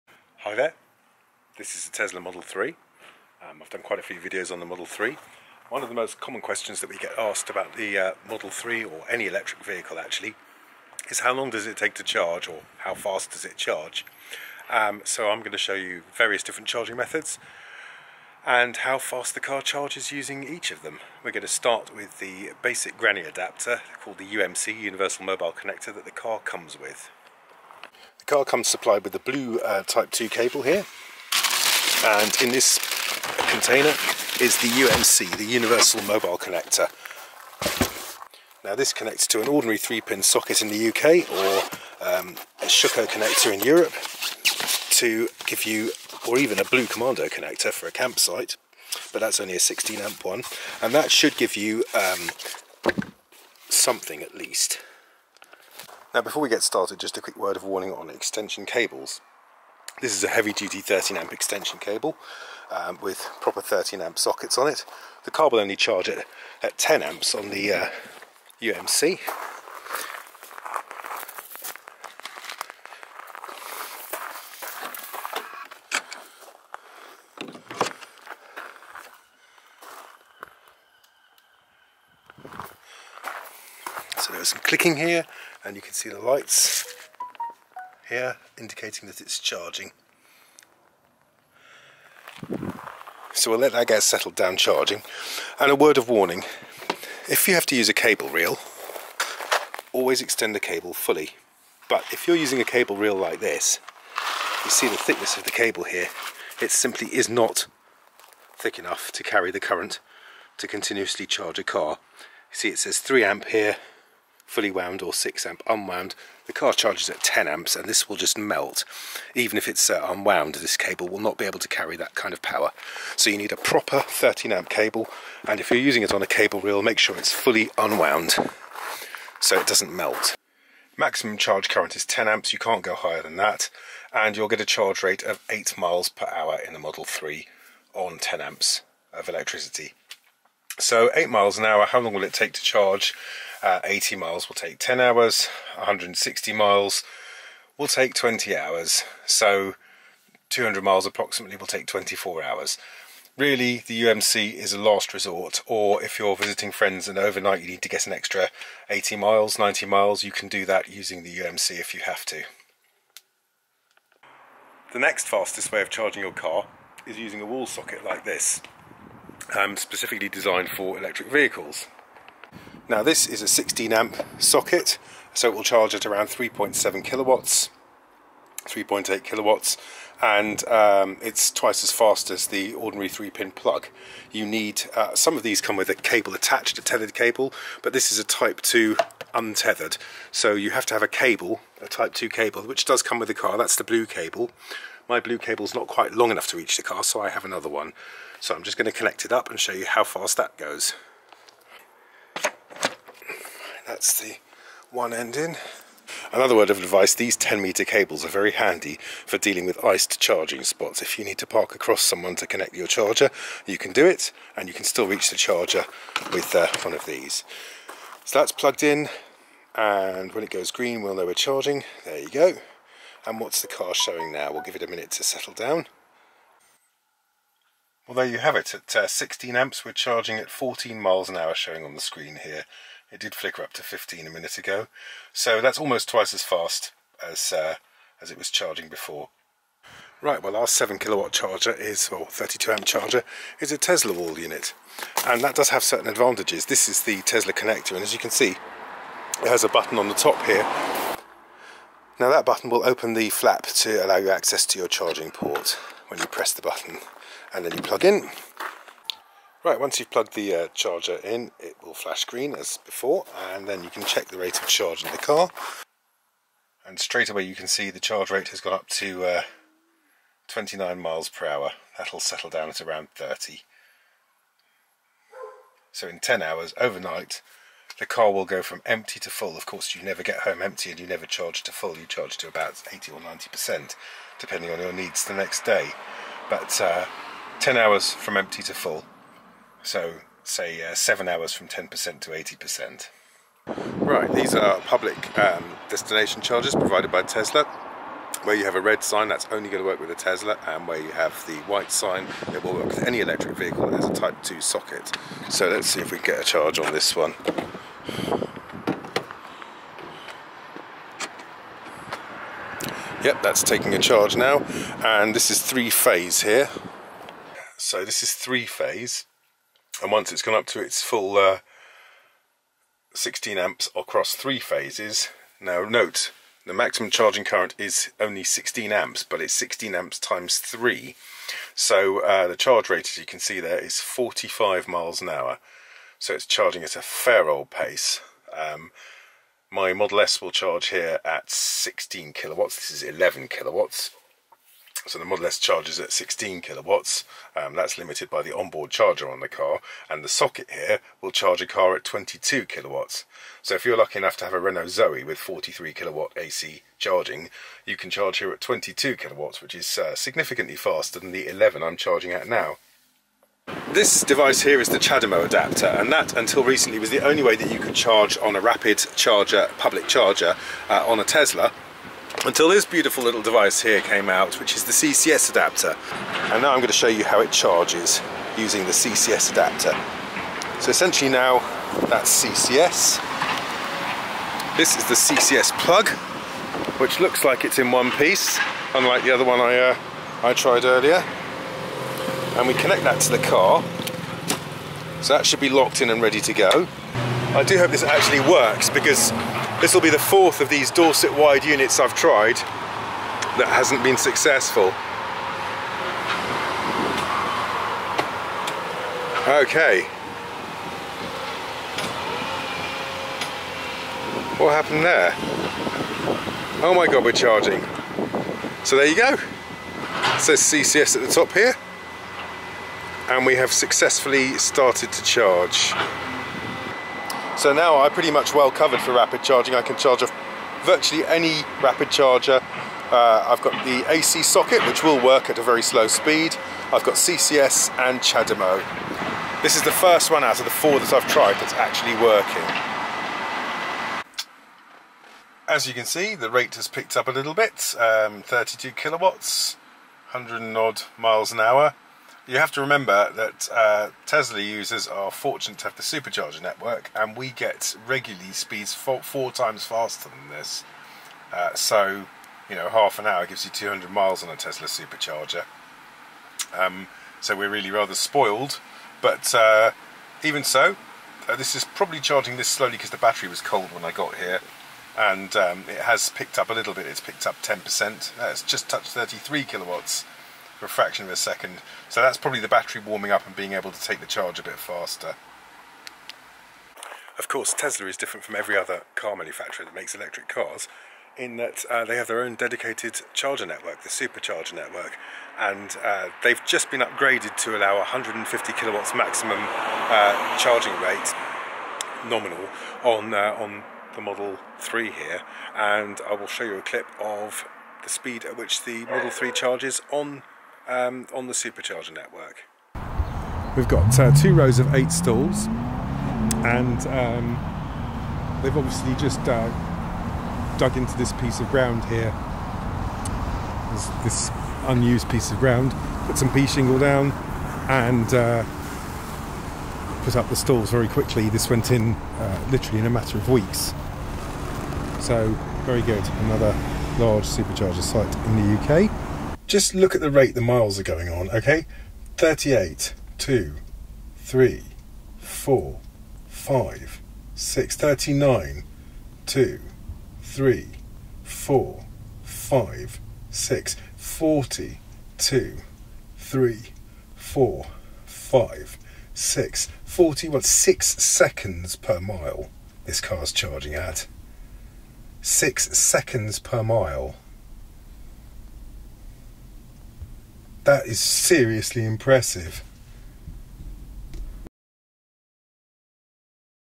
Electric Vehicle Charging 10 minute guide - how fast, how long does it take, how far can you go?
We have a look at many of the options available for charging your electric vehicle, from the slowest domestic power outlet on a 3 pin plug using your charging brick / evse / UMC adapter, through home wall box options, out into public destination charging, and public rapid charging, using Chademo and CCS DC Rapid units, before we finally look at the Tesla Supercharger sites. All these tests were done on a Tesla Model 3 Long Range UK model, except the public rapid chrager was done on a Tesla Model S 100D. All except the SuperCharger and the red Tesla Destination Charger will work with any electric vehicle.